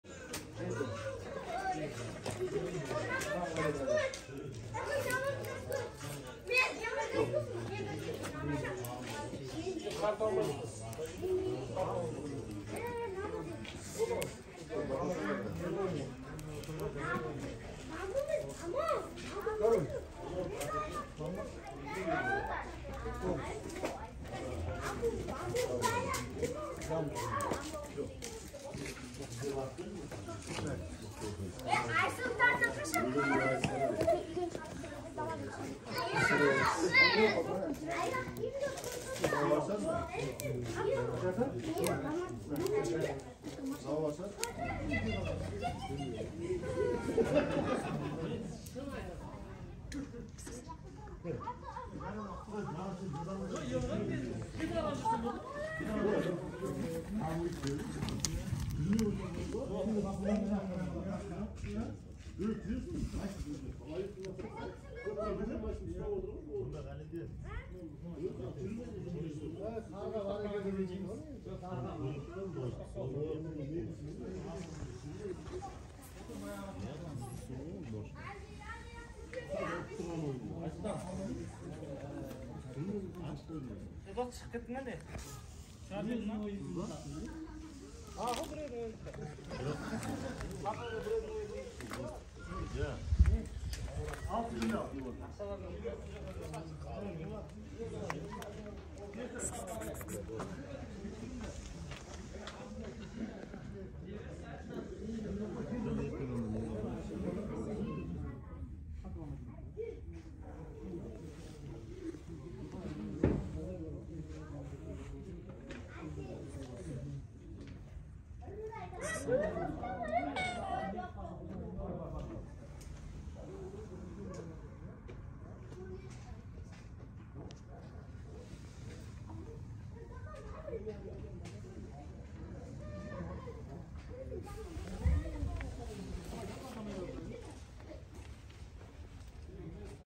Beautiful evening, barbecue, and a beautiful cat
Good evening, barbecue, and a beautiful cat